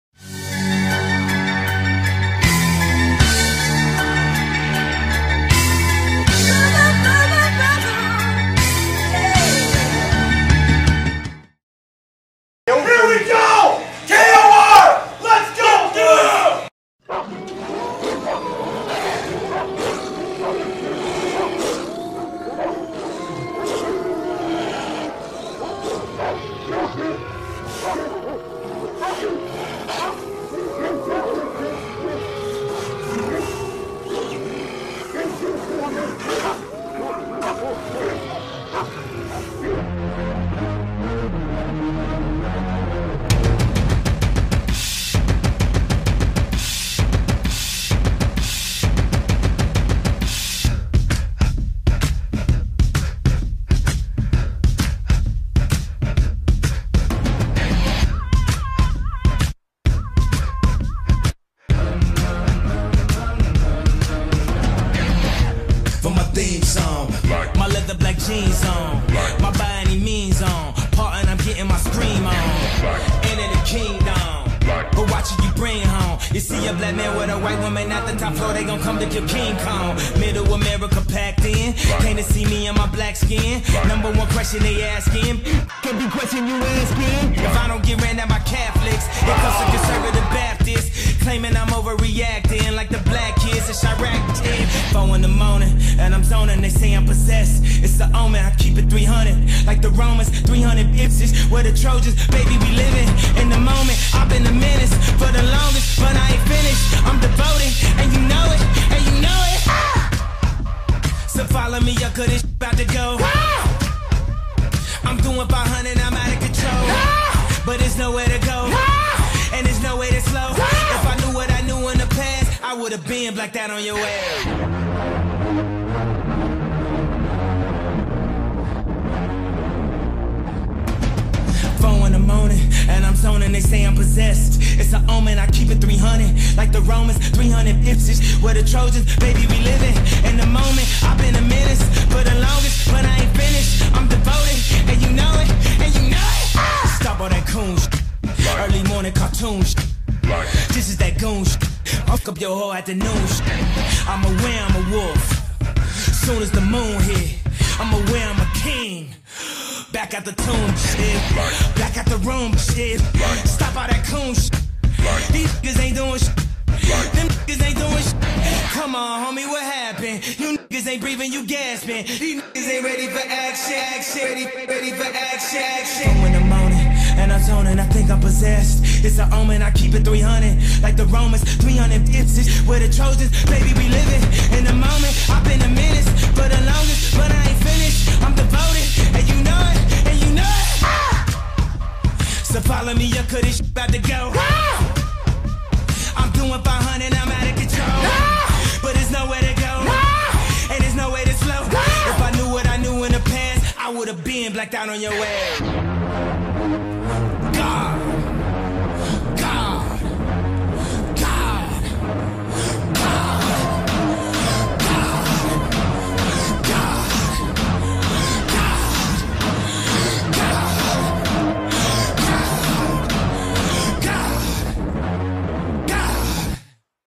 Stingers Football Highlight Film // 2013
Check out the Stingers Football highlights from the 2013 season.